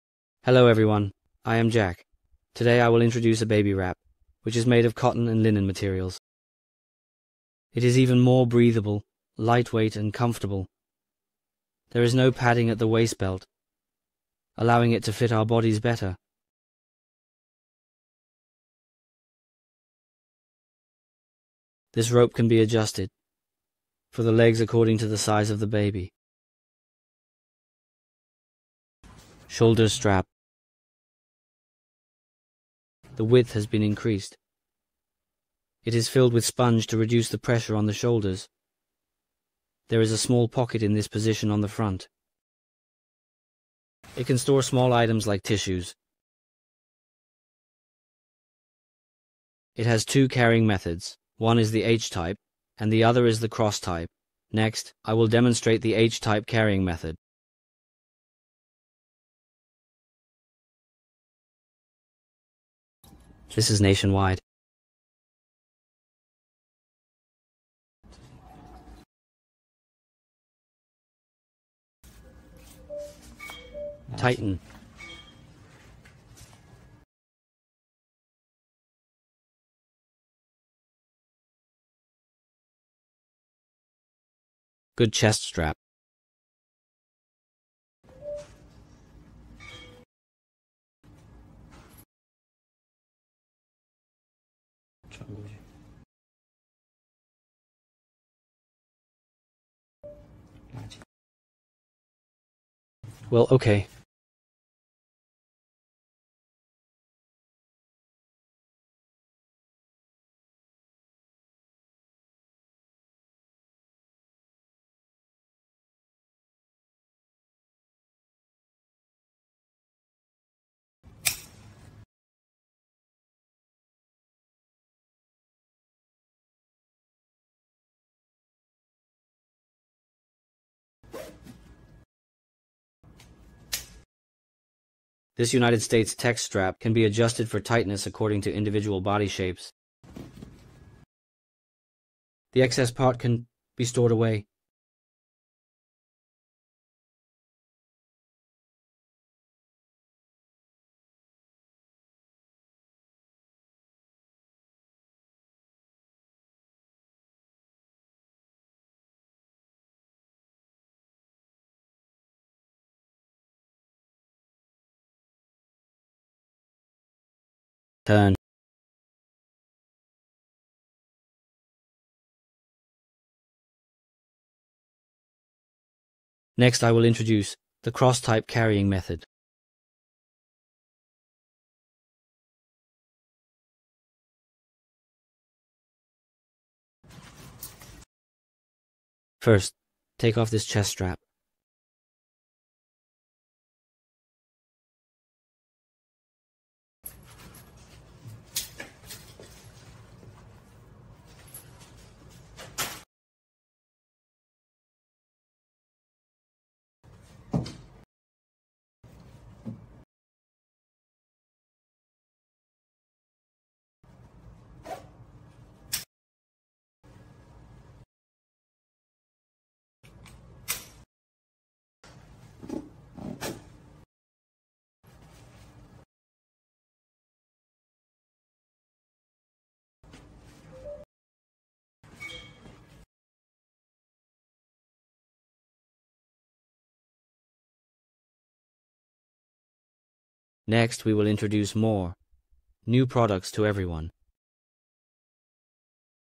Introducing the Perfect Blend of Comfort and Style: Cotton Linen Baby Carrier!
Discover a baby carrier that grows with your little one! 🌱 Our cotton linen baby carrier is designed to provide unmatched comfort for babies aged 6 to 36 months. Crafted with high-quality, breathable fabric, it keeps both you and your baby cool during every adventure. 🎒✨ Perfect for families on the go—whether you're out for a walk or tackling everyday tasks, this carrier has got your back (and your baby’s too 😉).